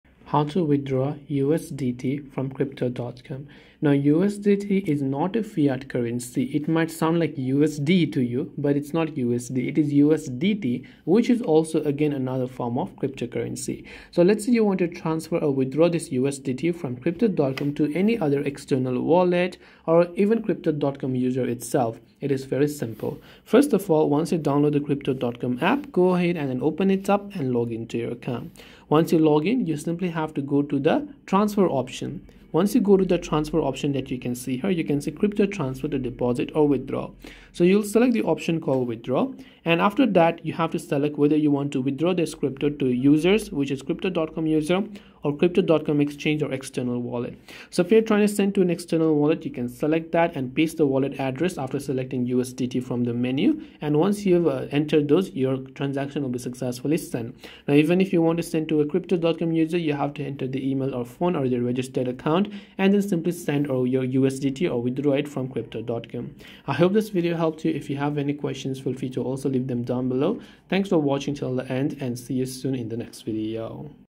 How to Withdraw USDT from Crypto.com !
To withdraw USDT from Crypto.com, whether to an external wallet or another Crypto.com user, follow these steps. Start by launching the Crypto.com app and logging into your account. Once logged in, navigate to the "Transfer" option visible on the app’s main interface.

In the transfer menu, select "Withdraw." Next, you’ll be prompted to choose between withdrawing to another Crypto.com user, Crypto.com Exchange, or an external wallet. If you’re transferring to an external wallet, select that option, then paste the recipient’s wallet address. Ensure that the wallet address is correct and supports USDT. Choose USDT from the cryptocurrency menu and confirm the transaction.

After entering the required details, confirm the transaction. The withdrawal will be processed and sent to the specified destination.

Timestamps:
0:00 Introduction
0:04 Understanding USDT
0:14 Launching the Crypto.com app
0:24 Navigating to the "Transfer" option
0:32 Selecting "Withdraw"
0:41 Choosing the withdrawal destination
0:49 Sending to an external wallet
1:00 Sending to another Crypto.com user
1:13 Conclusion